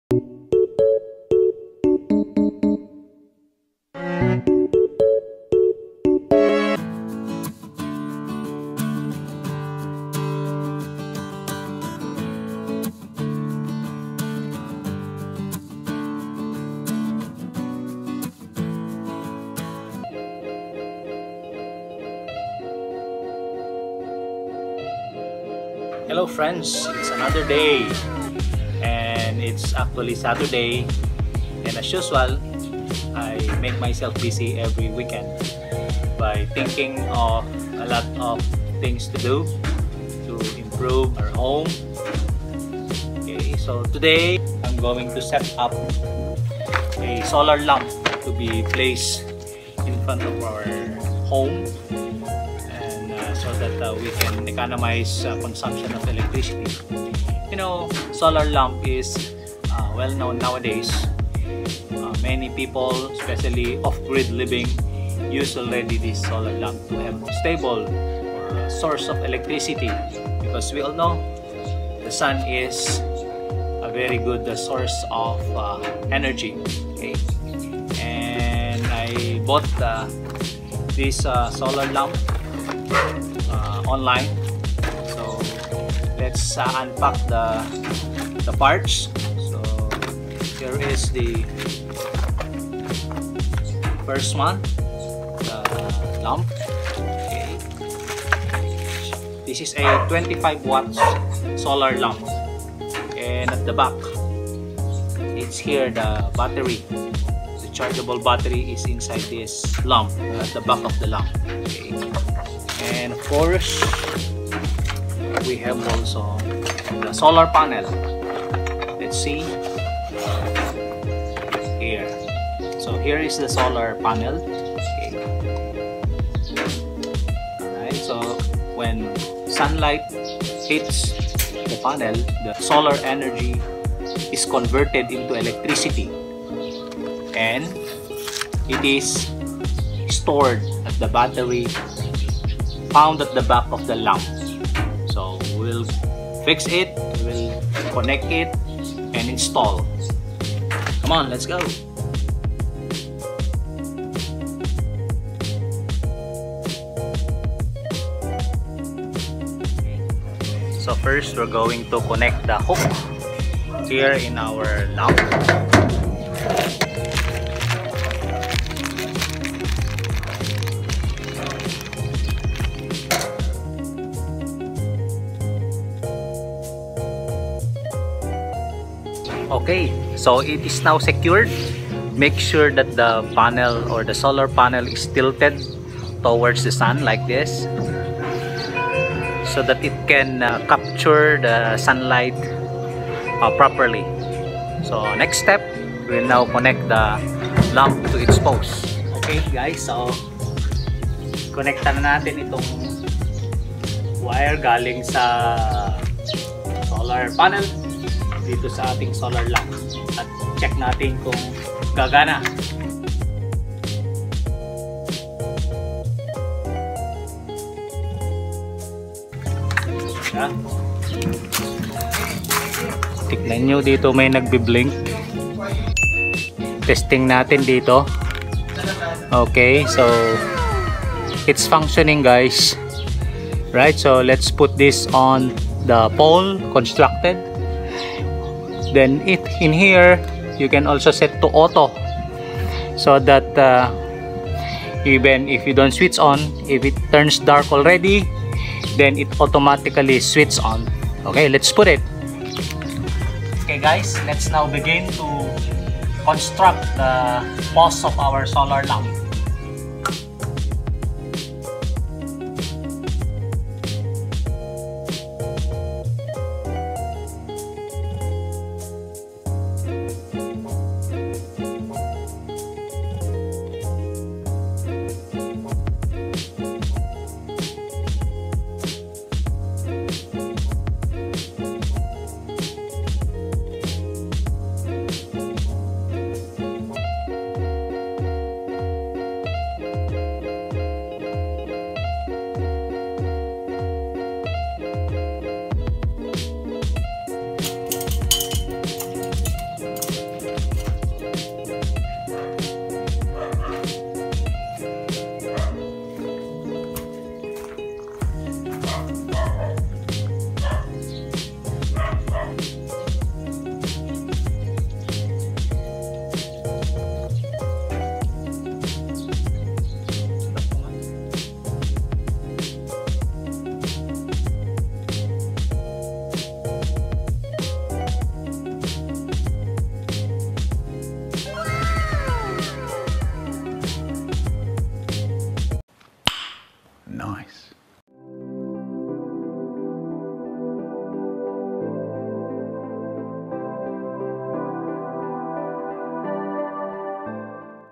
Solar Lamp Installation
The sun is an enormous source of energy that can be harnessed and be converted to electricity. Green technology is now becoming popular. It helps people especially people living off-grid in having electrical supplies in their homes. This source of energy is renewable which does not harm the environment.